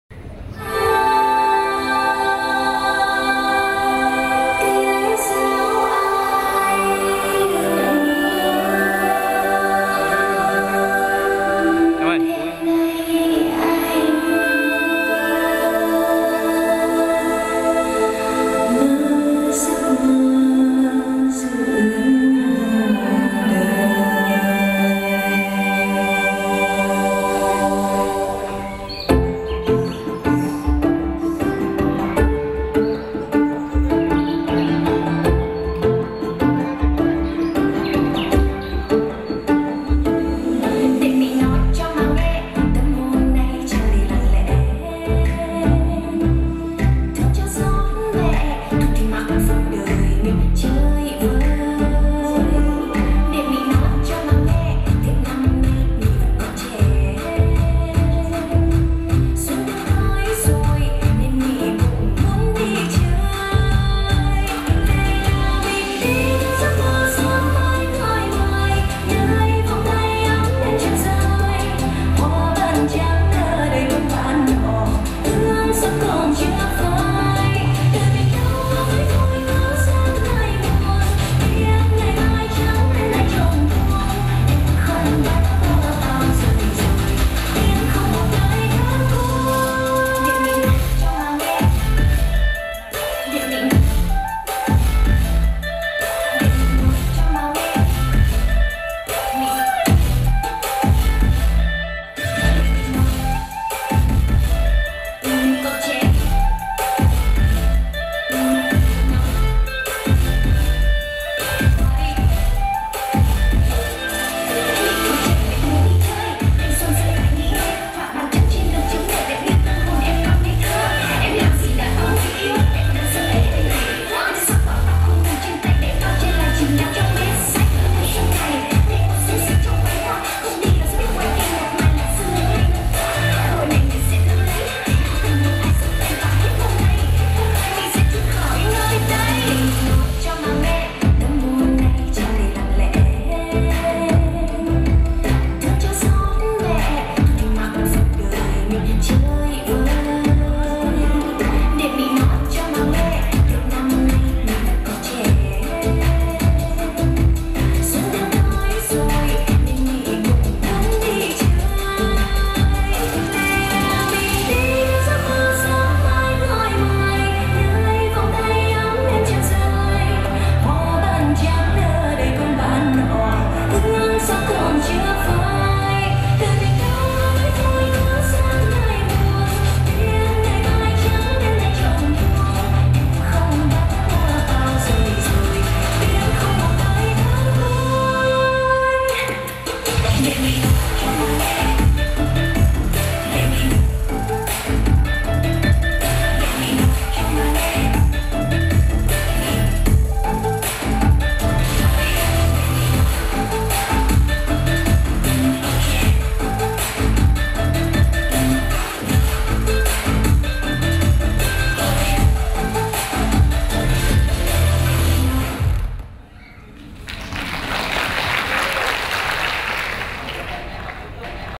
Tiết mục múa của công nhân nhà máy Yamaha - Nội Bài
phần trình bày của công nhân nhà máy Yamaha - khu công nghiệp Nội Bài - Hà Nội